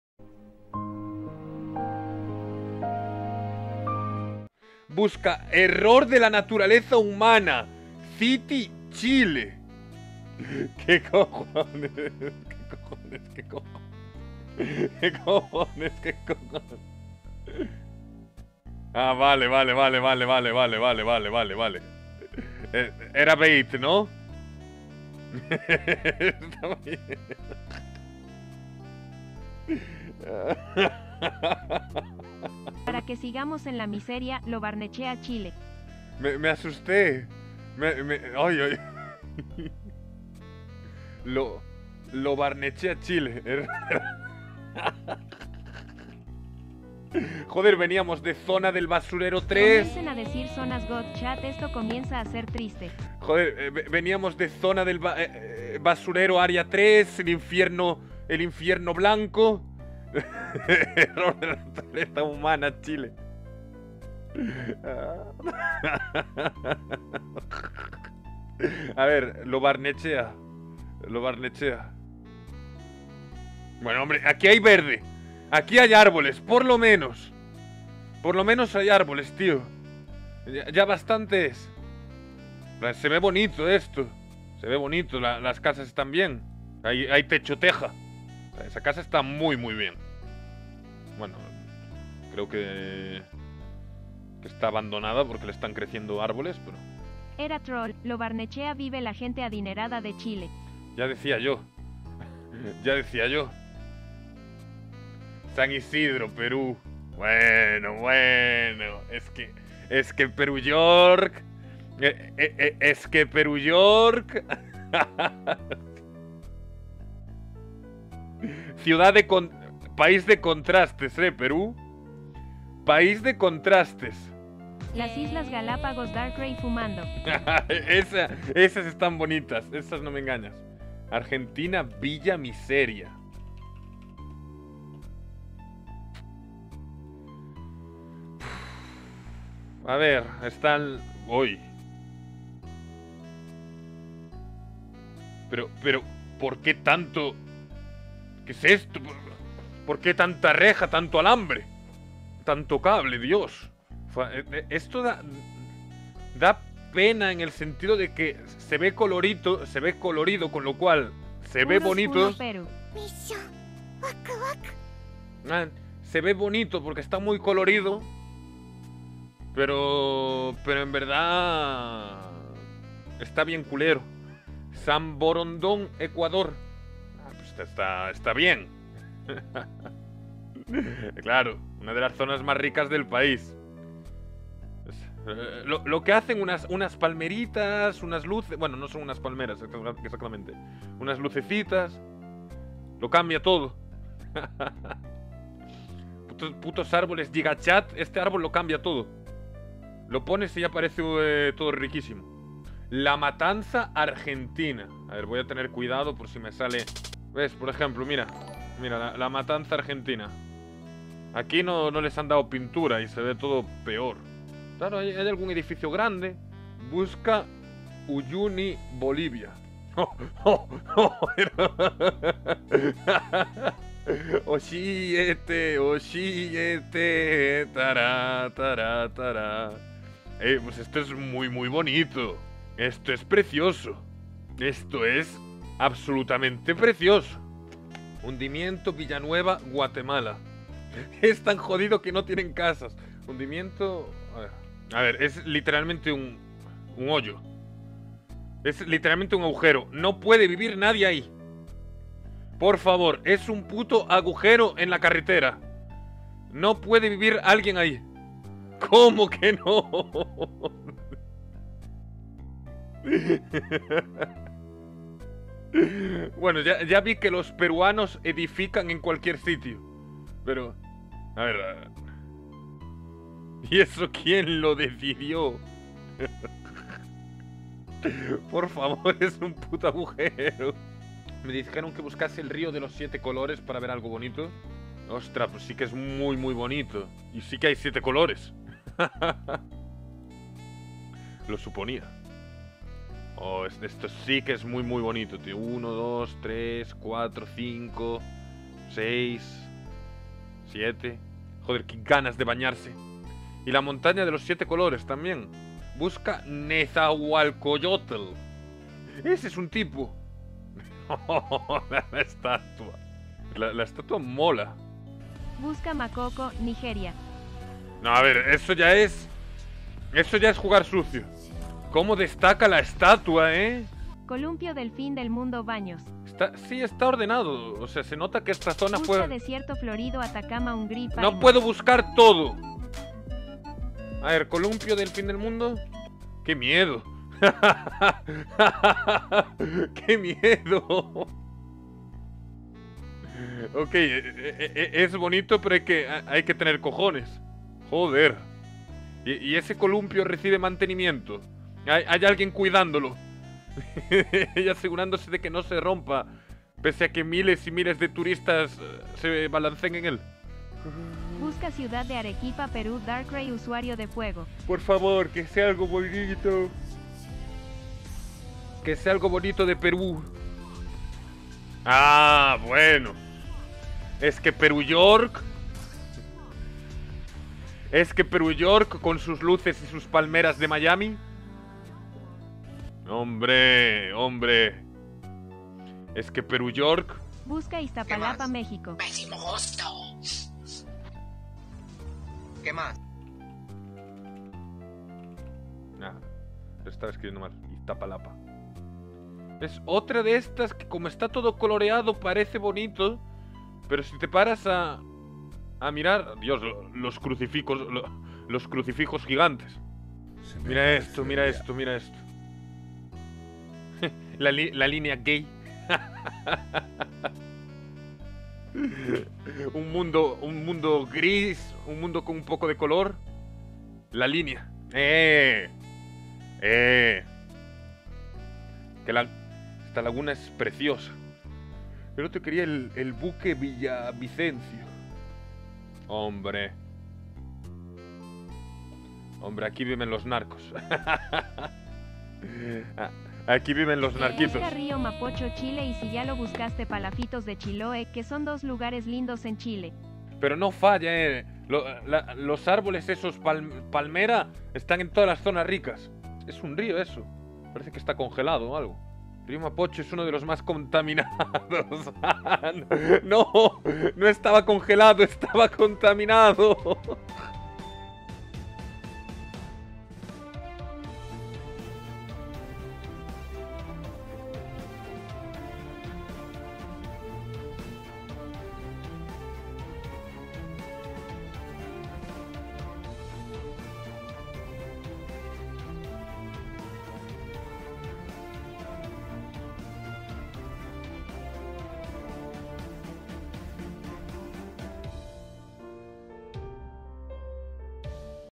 DARKRAI REACCIONA A LOS LUGARES MÁS POBRES DE LATINOAMÉRICA #2
Español reacciona- Darkraimola reacciona a las zonas mas techolamina de latam que le pasan sus seguidores pero con zonas bonitas